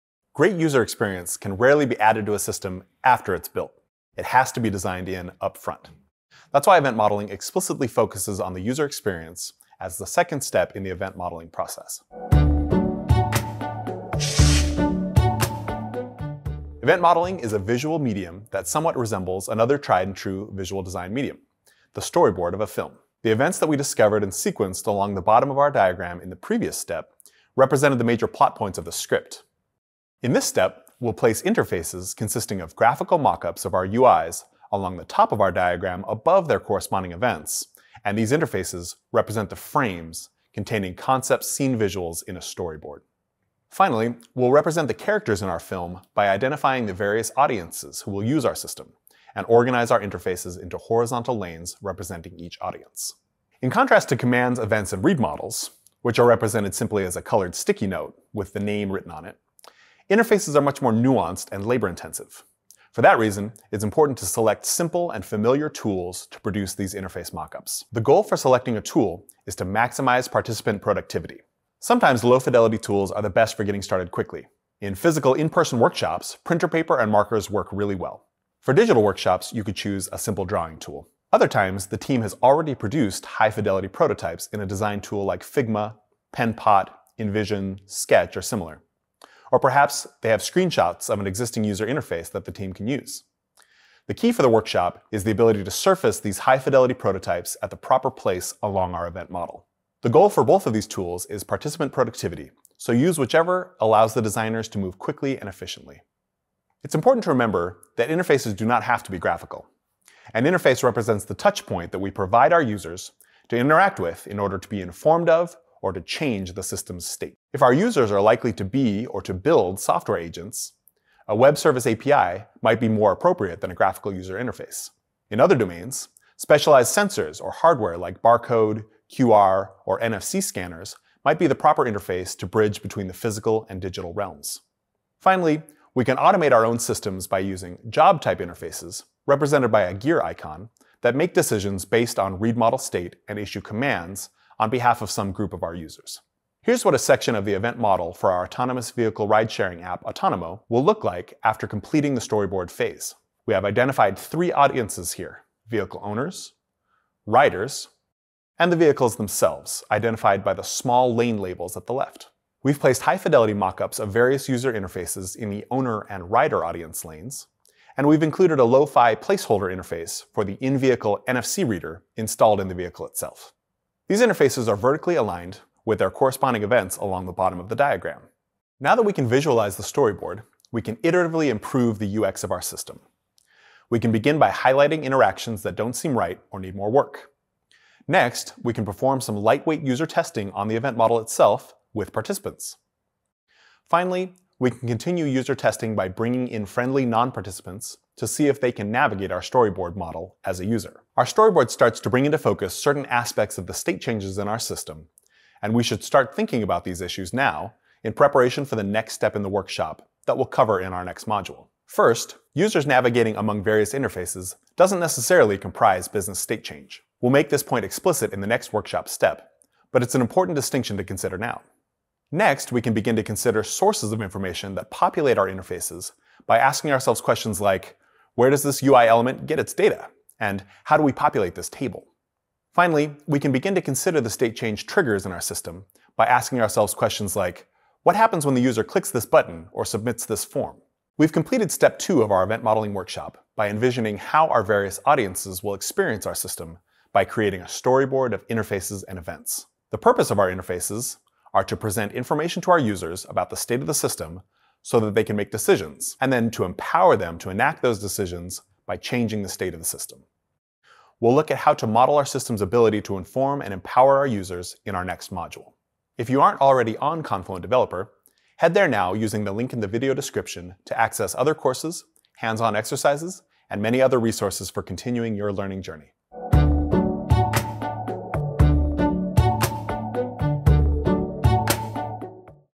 Event Modeling Step 2: Envisioning the User Experience
Great user experience can rarely be added to a system after it’s built, it has to be designed in up front.  That’s why Event Modeling explicitly focuses on the user experience as the second step in the Event Modeling process.  We'll envision the user experience using Interfaces that align with our Event narrative, creating something similar to a storyboard for a film.